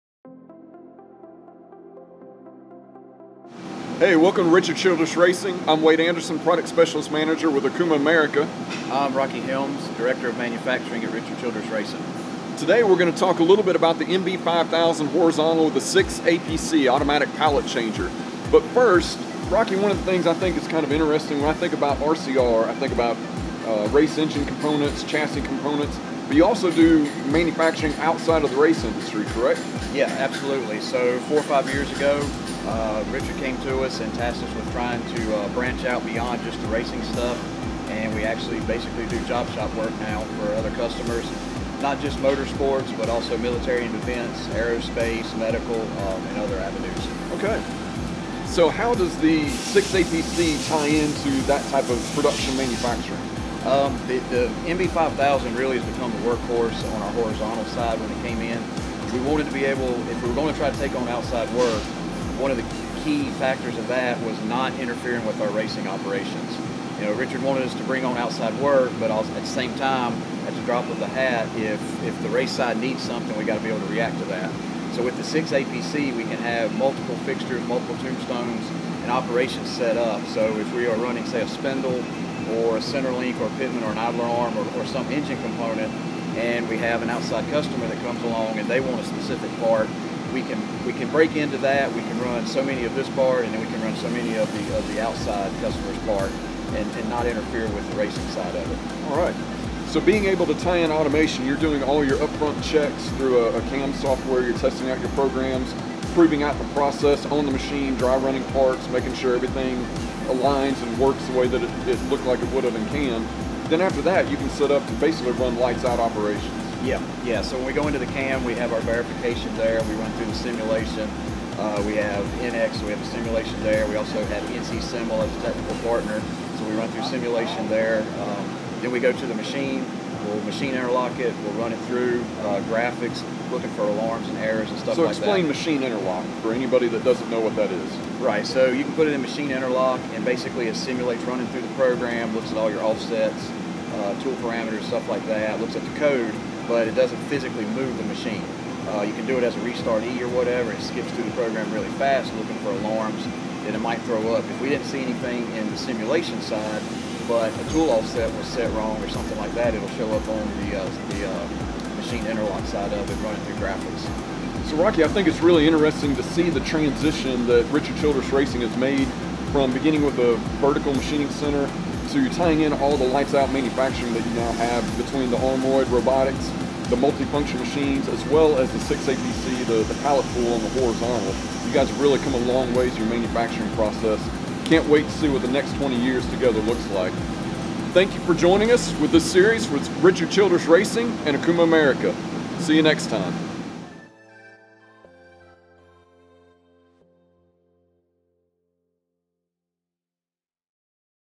Okuma and Richard Childress Racing - Running Lights-Out
From automated up-front checks in CAM software to testing out programs, the team at Richard Childress Racing are pros at running lights out on their Okuma machines. Learn about RCR’s machining process that makes them an efficient and high-precision shop!